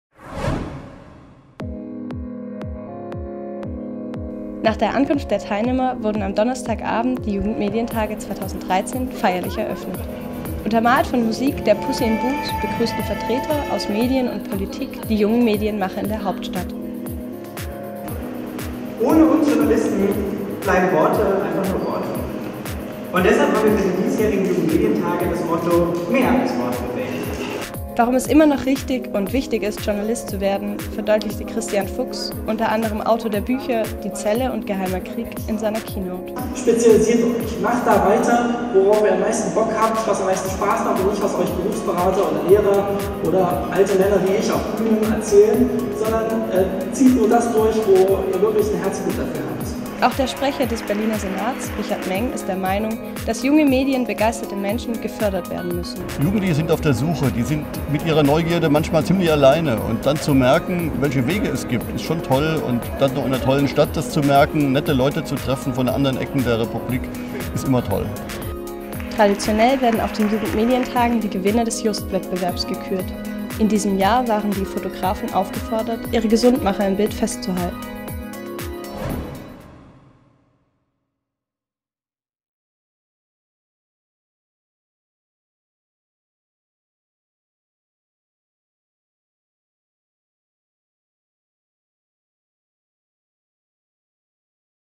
Jugendmedientage 2013: Auftaktveranstaltung
"Zieht nur das durch wofür ihr wirklich Herzblut habt" empfiehlt Christian Fuchs bei der feierlichen Eröffnung der Jugendmedientage am Donnerstagabend. Begrüßt wurden die Teilnehmer in der Hauptstadt von Vertretern aus Medien und Politik, sowie der Band Pussies in Boots.

Beitrag: Lea Goeppert, Julia Zablotny, Lars Ippich und Florian Schwarz.